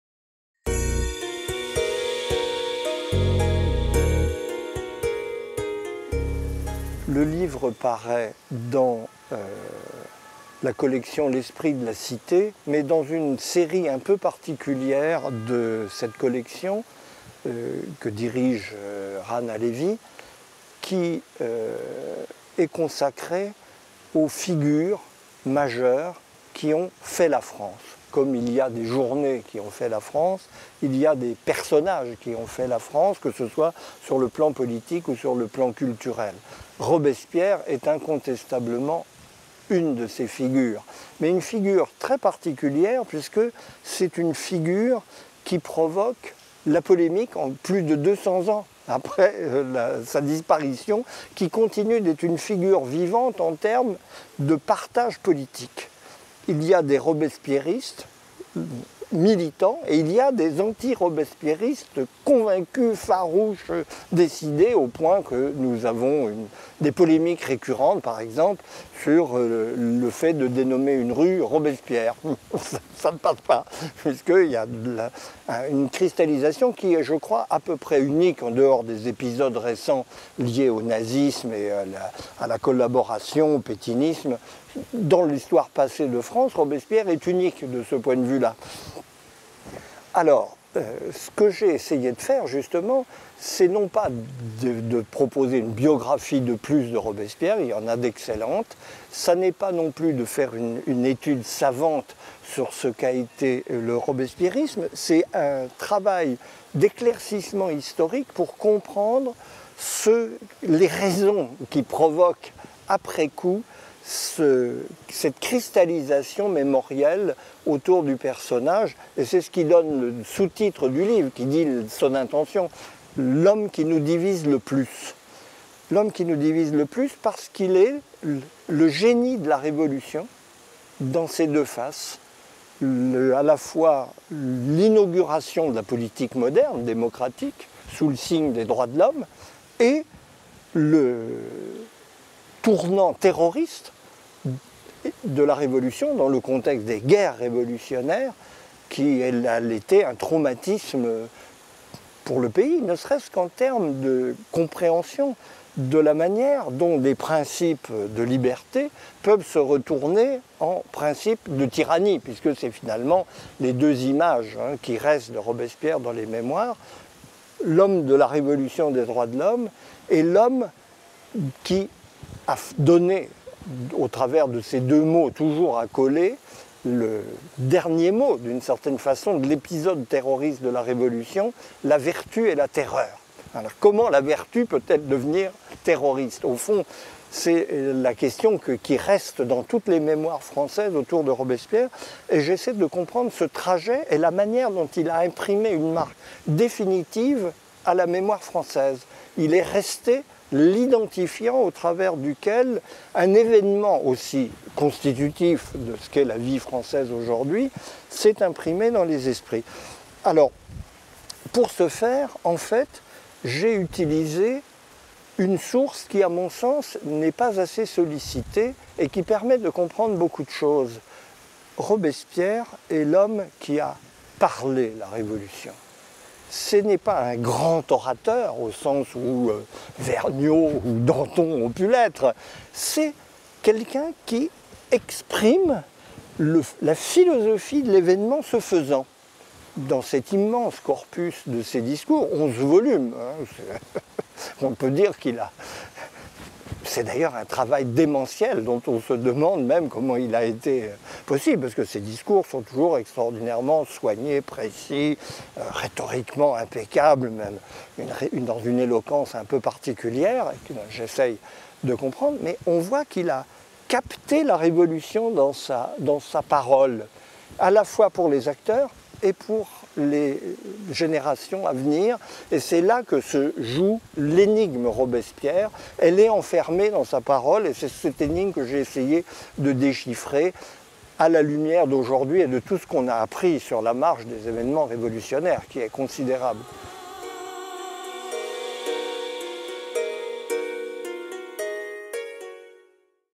Marcel Gauchet - Robespierre, l'homme qui nous divise le plus
Marcel Gauchet vous présente son ouvrage "Robespierre, l'homme qui nous divise le plus". Parution le 18 octobre 2018 aux éditions Gallimard. Rentrée Sciences Humaines automne 2018.